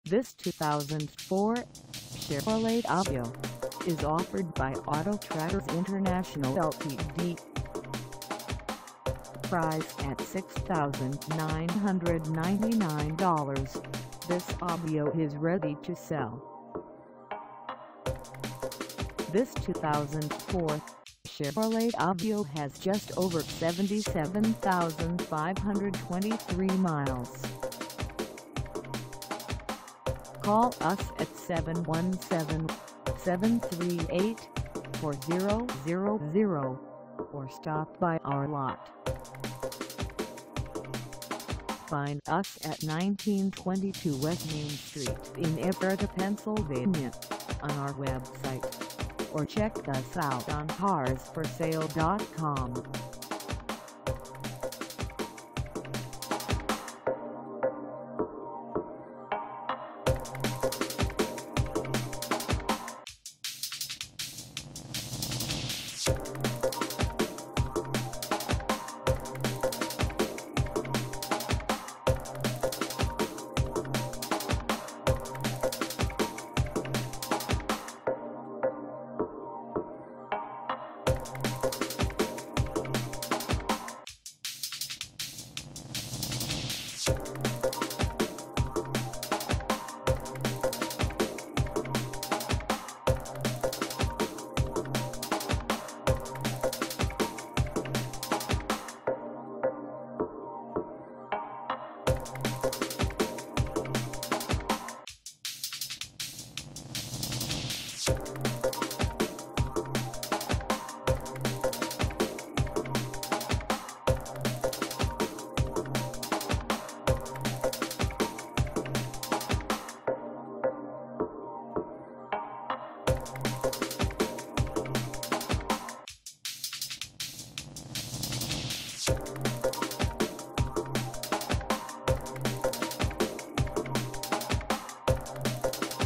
2004 Chevrolet Aveo LS for sale in Ephrata, PA 17522 at Auto
This 2004 Chevrolet Aveo LS is for sale in Ephrata, PA 17522 at Autotraders International LTD.
Engine: 4-Cylinder 1.6L 4 Cyl
Transmission: Automatic
Color: PEARL BLACK CLEARCOAT
Mileage: 77523
Contact Autotraders International LTD 1922 W Main St Ephrata, PA 17522 to test drive this 2004 Chevrolet Aveo LS today.